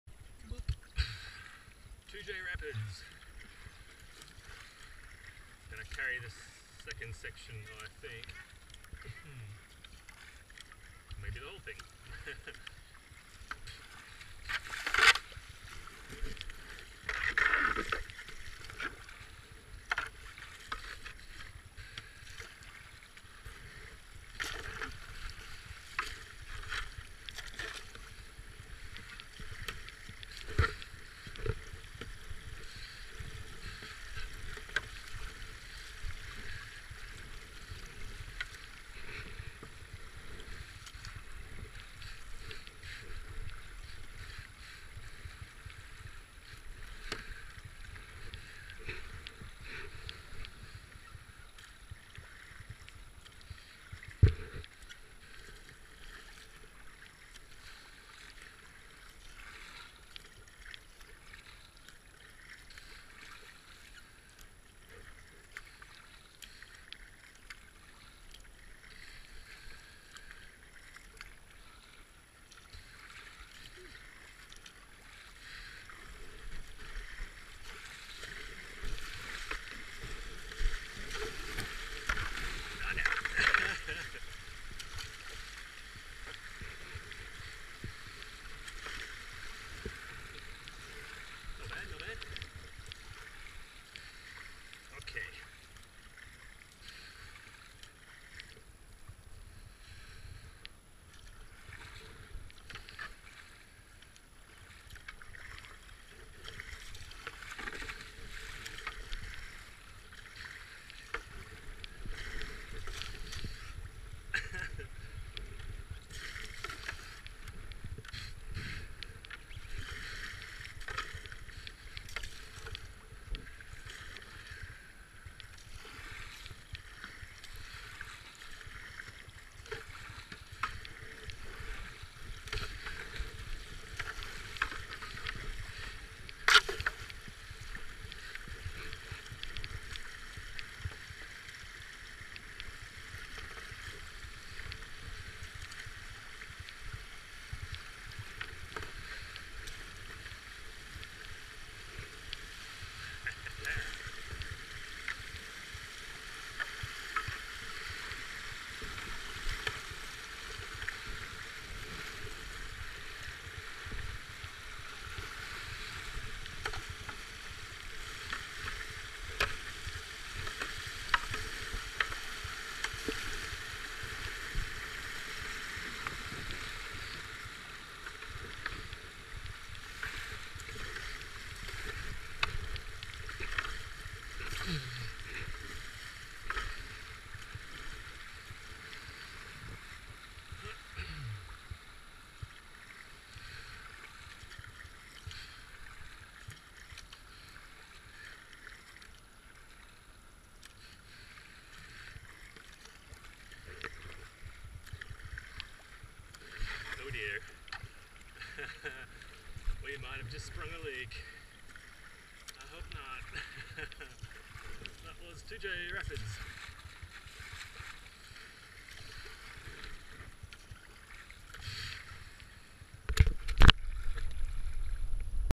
'Avon Descent' 2020 - 08 Day1 Toodyay Rapids
The official Avon Descent 2020 has been cancelled.. But that didn't stop 4 of us!

 This shows the little rocky rapids soon after the centre of Toodyay that I like to call Toodyay Rapids (doesn't actually have a name).
There is usually a photographer at the bottom catching all the spills and thrills.. Or all the swearing and bruising! Crunch Crunch Crunch!

Predicted Water Level was 0.02m in the TiTrees and 0.29m in the Valley, with actual levels closer to 0.2m in TiTrees and 0.26m in the Valley.